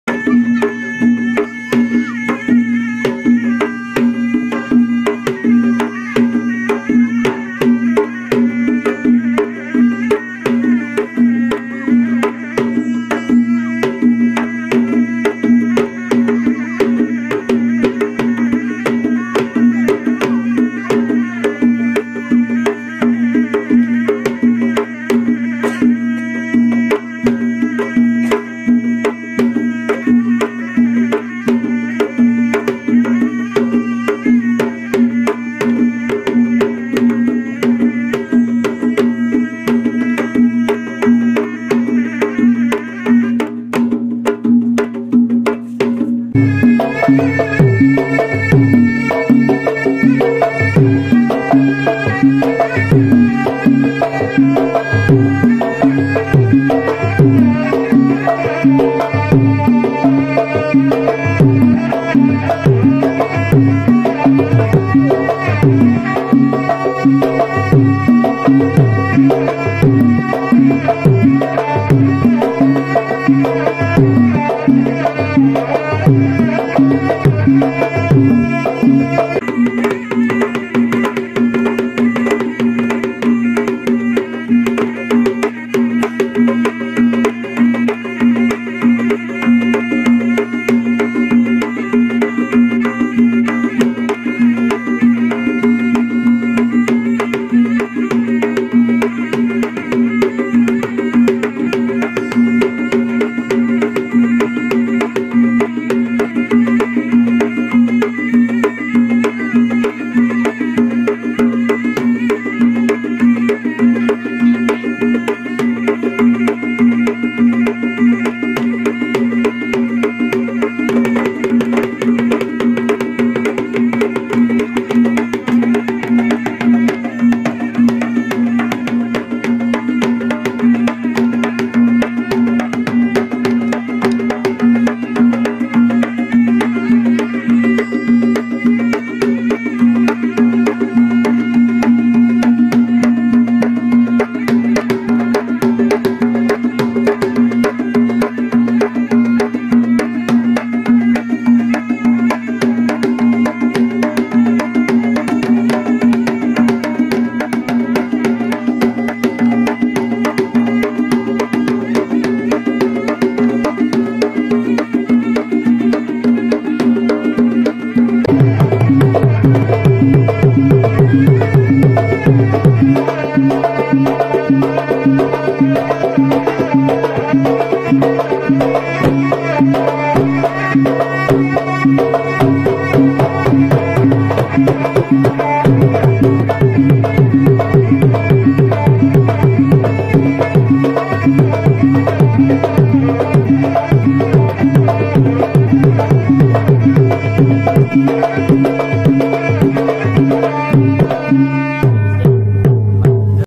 Kendang Instrument#
Kendang or Gendang is a two-headed drum used by people from the Indonesian Archipelago. Kendang is one of the primary instruments used in the Gamelan ensembles of Javanese, Sundanese, and Balinese, the Kendang ensemble as well as various Kulintang ensembles in Indonesia, Brunei, Malaysia, Singapore, and the Philippines.